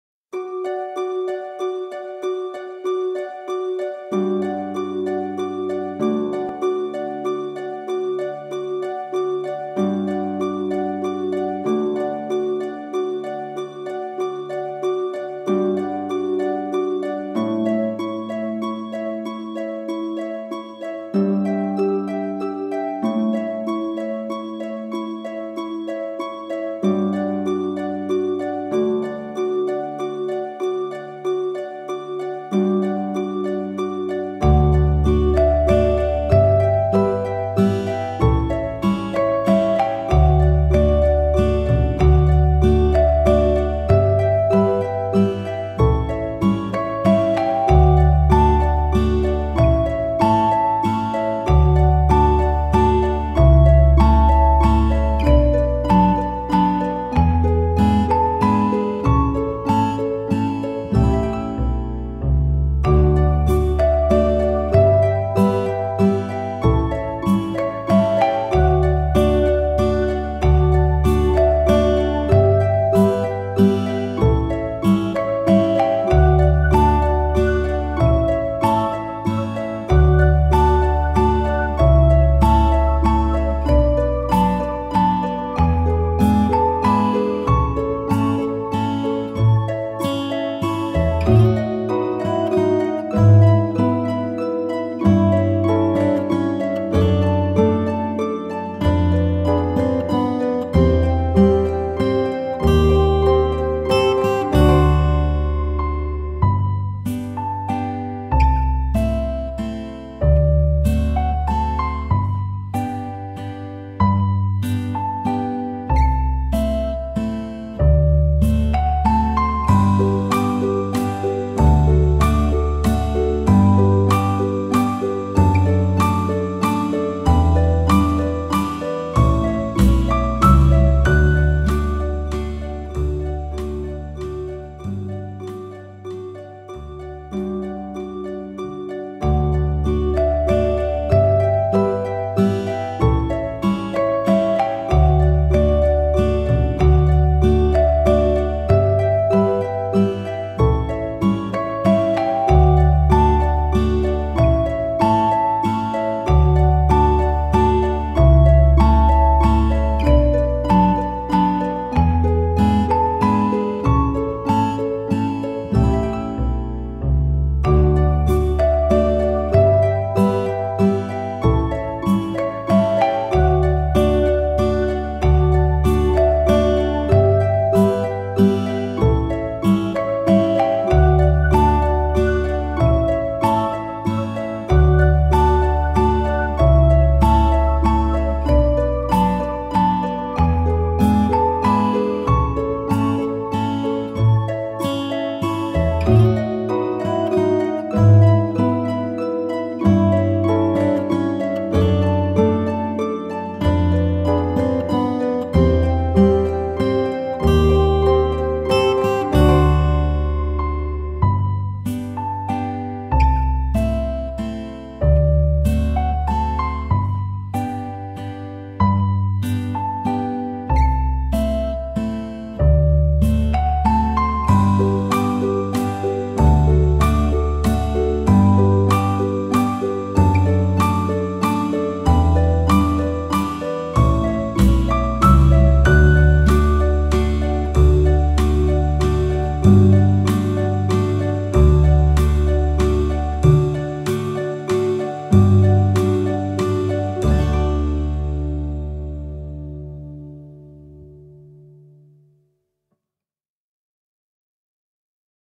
Happy Dance Music, Beautiful Life
Awesome Optimistic vibes Music, Relief from Stress and Anxiety, Enjoy beautiful nature, go dance in nature.
Enjoy beautiful music any time of the day.